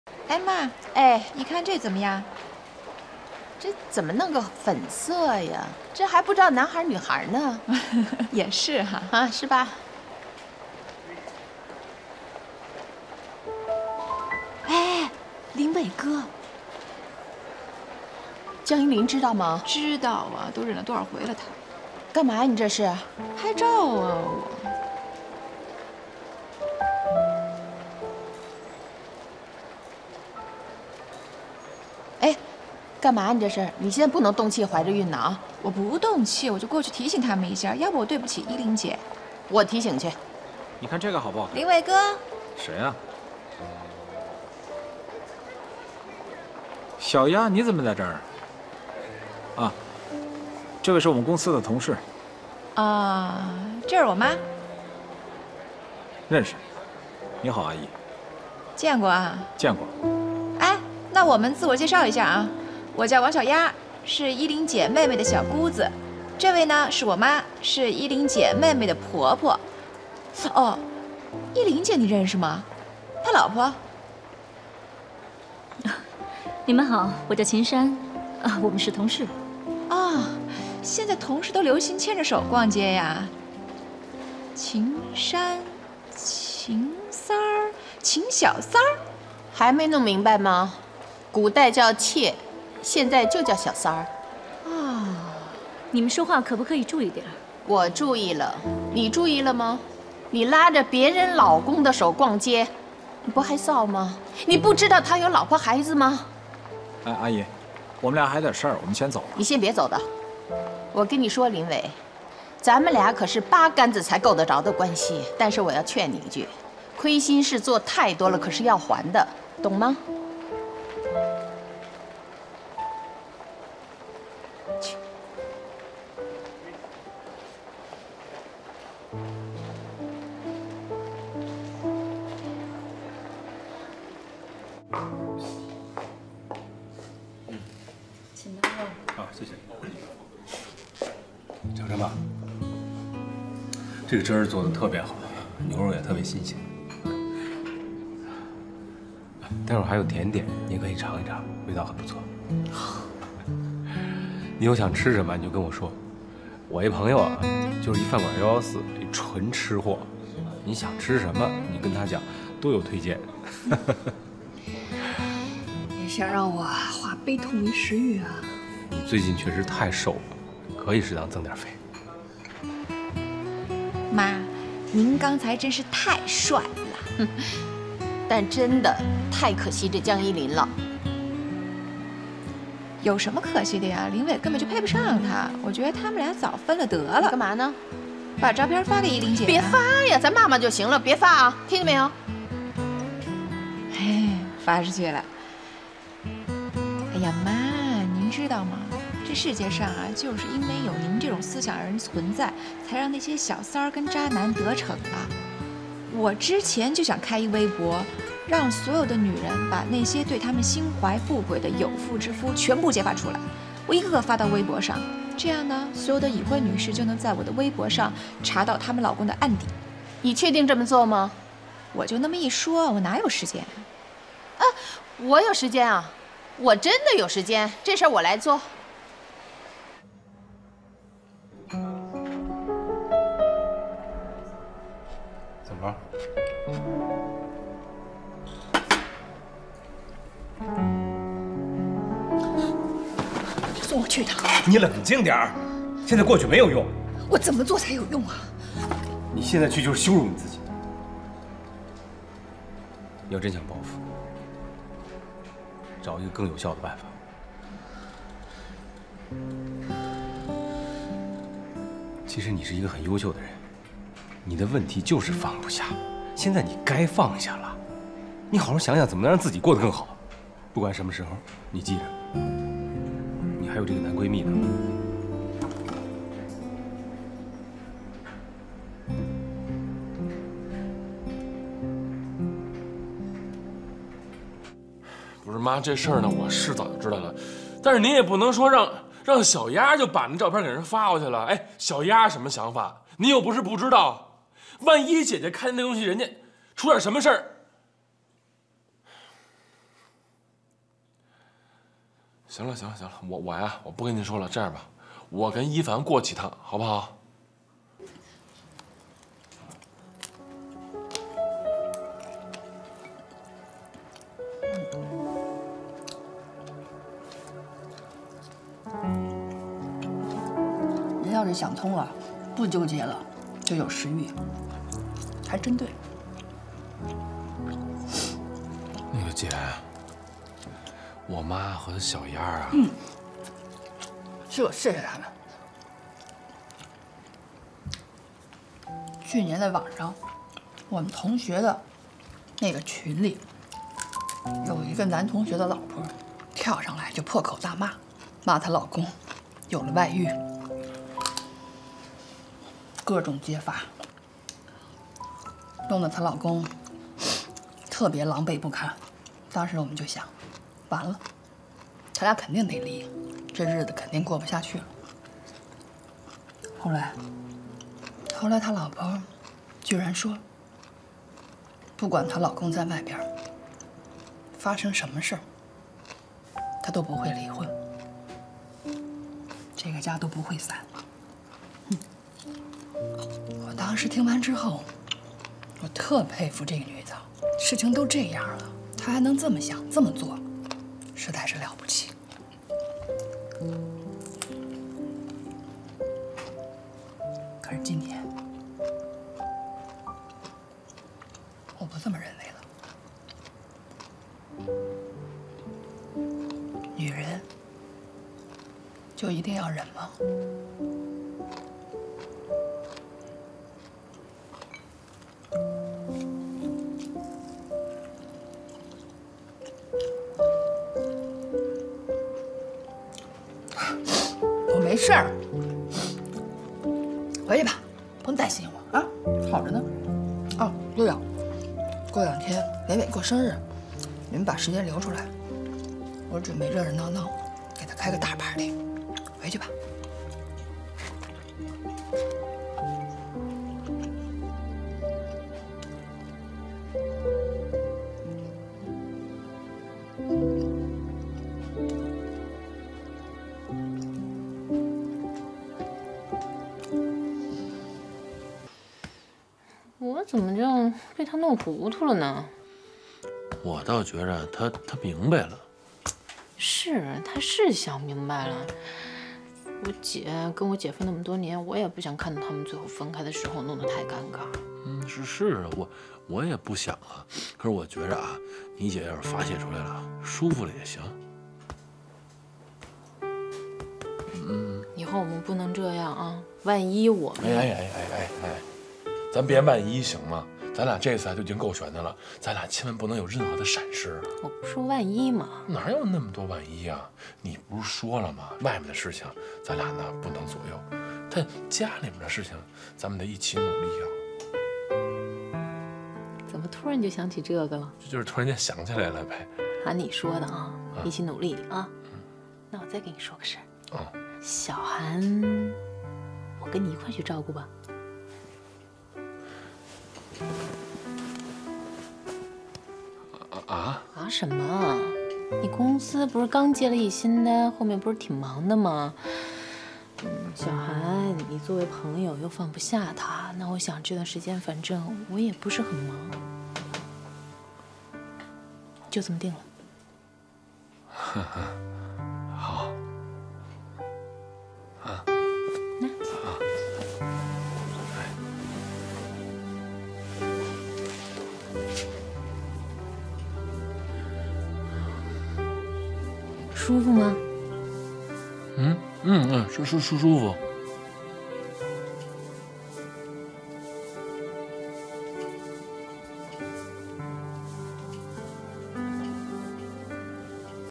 捉奸現場！丈夫和小三逛街，被妻子娘家人撞見，這下報應來了！
"24-jhyjxdd
💘【劇情簡介】該劇講述了老中青三代人在婚姻生活中不同的矛盾，以及在矛盾中不斷成長，最終各自收穫幸福的故事